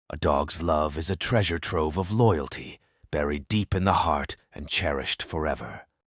Treasured Love: Dogs as Loyal Companions
Unearth the treasure trove of loyalty found in a dog's love, buried deep in the heart and cherished for eternity. Inspirational quotes about dogs and loyalty.